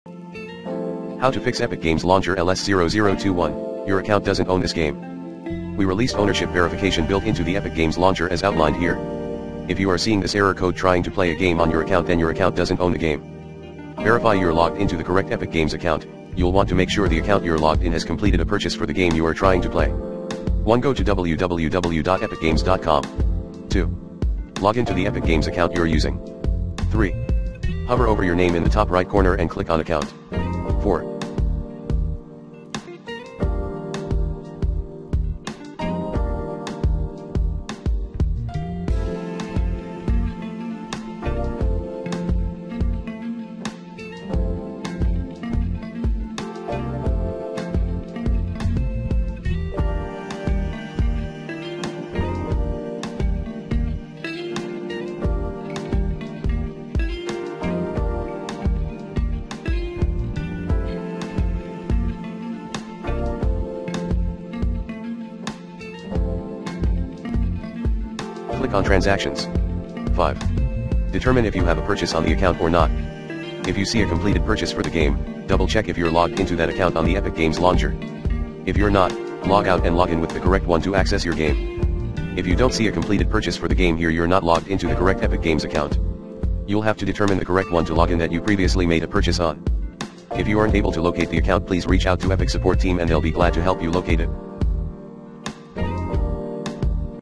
Fix Epic Games Launcher LS-0021: Your account doesn't own this game
Here's how to Fix Epic Games Launcher LS-0021: Your account doesn't own this game.